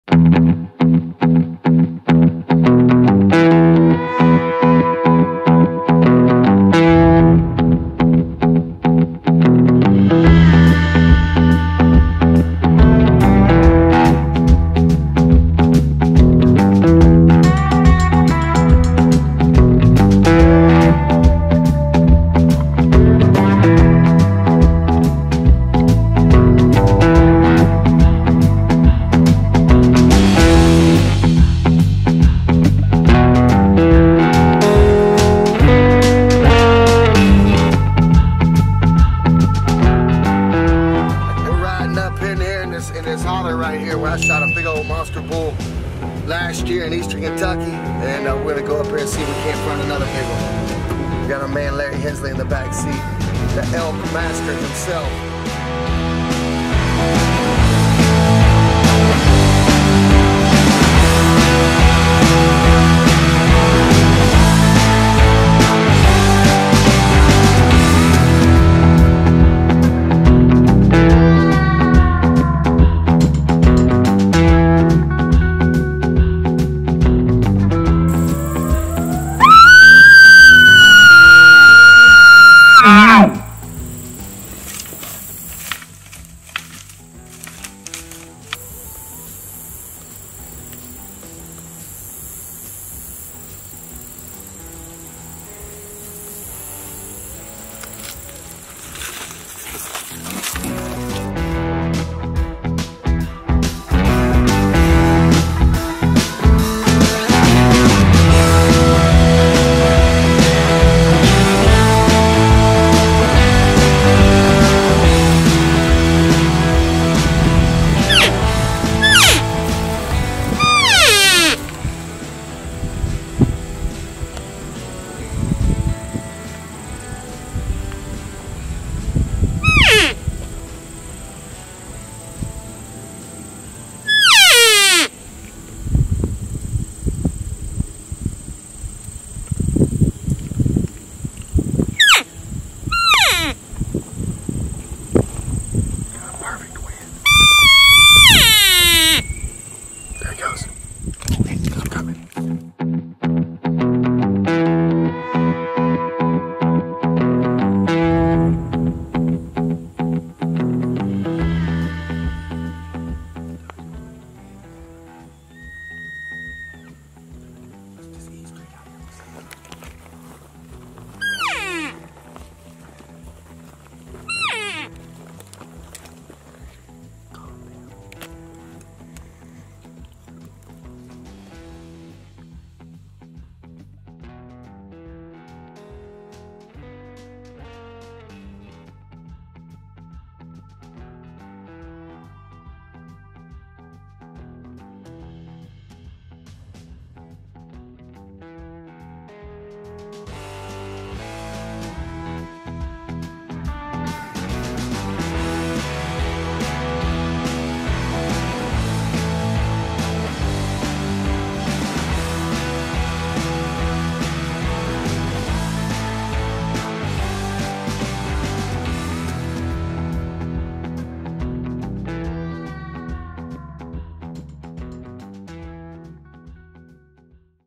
ELK COMES WITHIN 30 FEET | WHITETAIL HEAVEN OUTFITTERS
Elk season is always a great time, especially when a big bull hits the ground! We've been locating ELKs for our clients and we are pumped to share with you some of the elk hunts that are happening

Stay tune for more pictures